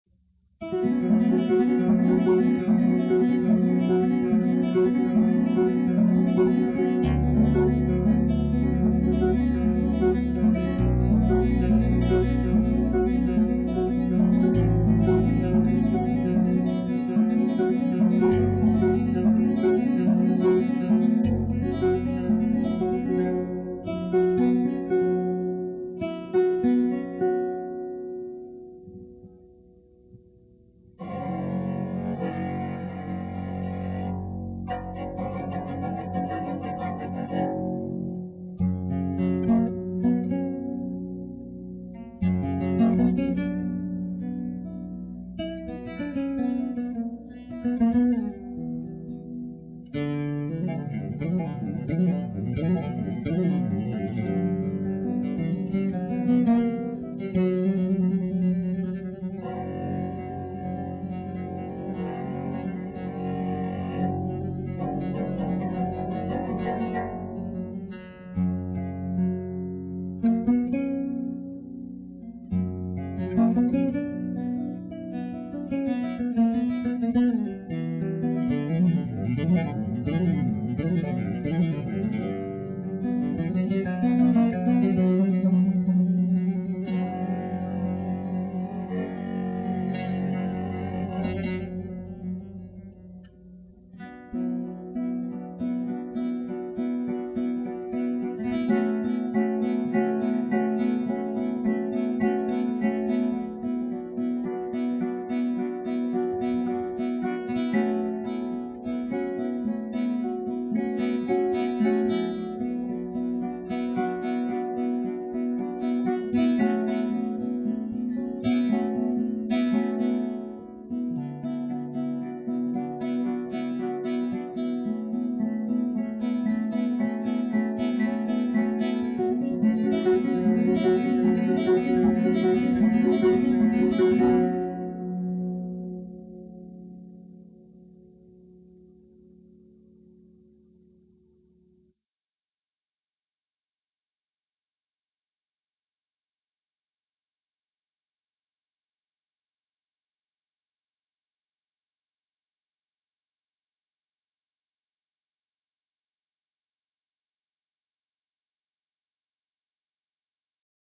Esteban Antonio
Esteban Antonio playing his unique Ha-Shem...watch how this folds down at the end...